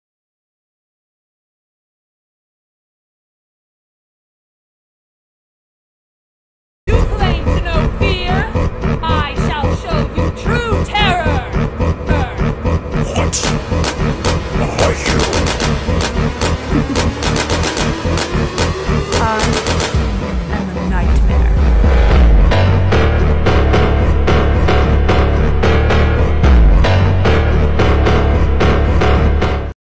I am veronica..~{new character}~{my au}~{TW: flasing lights!}~{READ DESC!}~
Hi guys hope you like this video also in my au vanny has a best friend she met in high school whos named veronica and sometime later they meet again and they became close besties and started telling there secrets bc they trusted eachother! So vanny told veronica that she was a murderer but turns out veronica was as well bc instead was inspired of the “white murderer bunny” aka vanny so after that veronica and vanny have been hanging more out since if this doesnt make sense to you thats fine! If you dont like my au thats fine as well! Over all i hope you have a great day and pls no hate comments pls ^^ 💕love u all 🫶🏻